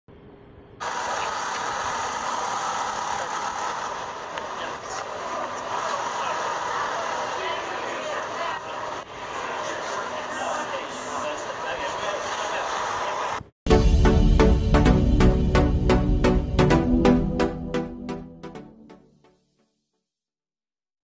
BREAKING NEWS: Former Trump lawyer Michael Cohen seen entering FBI New York office
Former Trump lawyer Michael Cohen seen entering FBI New York office as he is expected to plead guilty to charges stemming from a federal investigation in New York